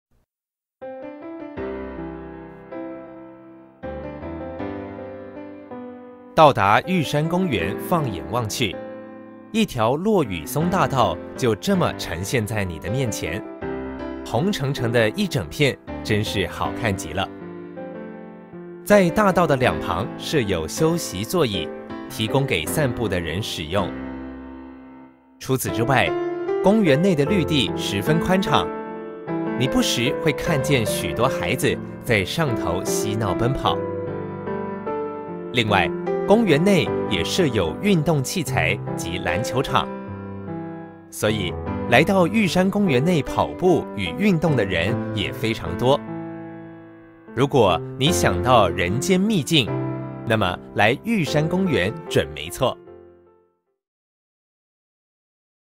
[桃園區] 玉山公園 (景點)
到達玉山公園望眼過去，一條落雨松大道就這麼呈現在你的面前，紅澄澄的一整片，真是好看極了！在大道的兩旁設有休息座椅，提供給散步的人使用，除此之外公園內的綠地十分寬敞，你不時會看見許多孩子在上頭嬉鬧奔跑，而且公園也有運動器材、籃球場，所以來到玉山公園內跑步與運動的人也非常多。如果你想到人間秘境，那麼來玉山公園準沒錯！